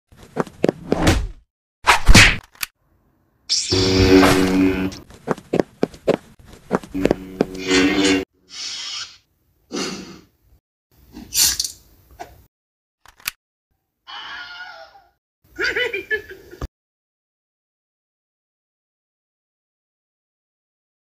3 Zombie Snowtroopers Attack Obi-Wan Kenobi | Lego Stop Motion
A short video I made of zombie snowtroopers attacking Obi-Wan.